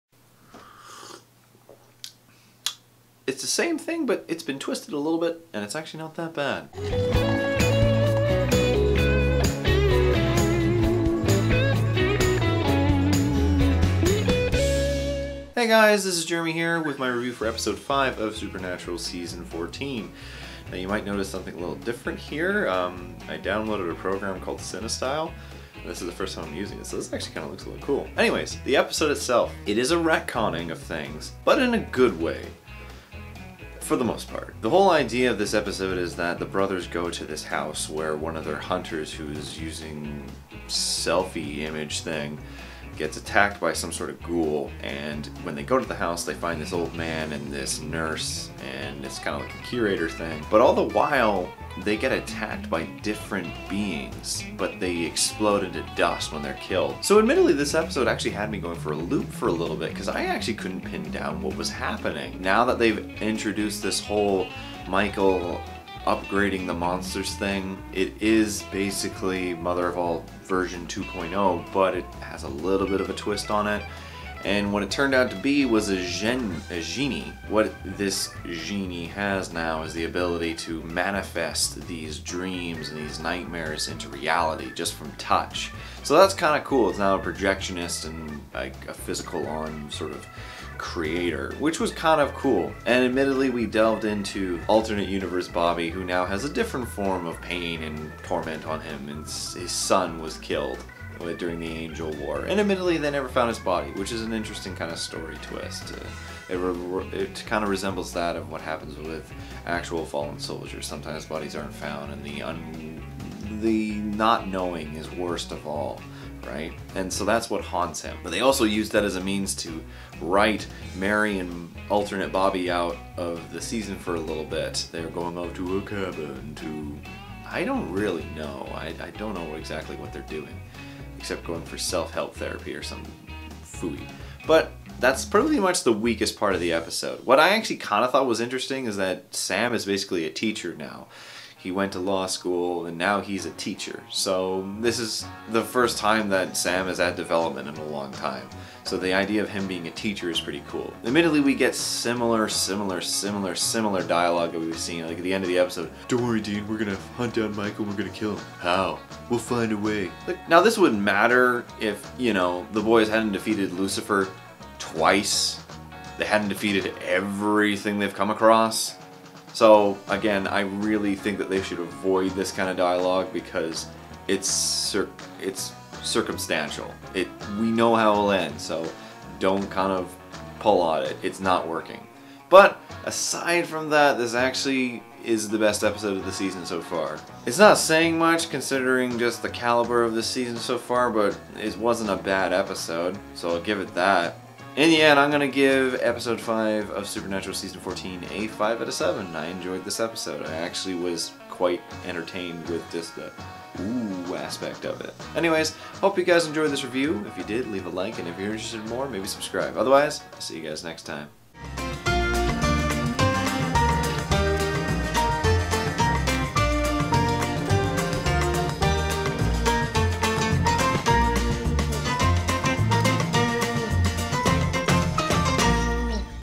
Supernatural Season 14 Episode 5 Review
Let's take a look at the 5th episode of Supernatural Season 14 and see if this sinking ship can be saved.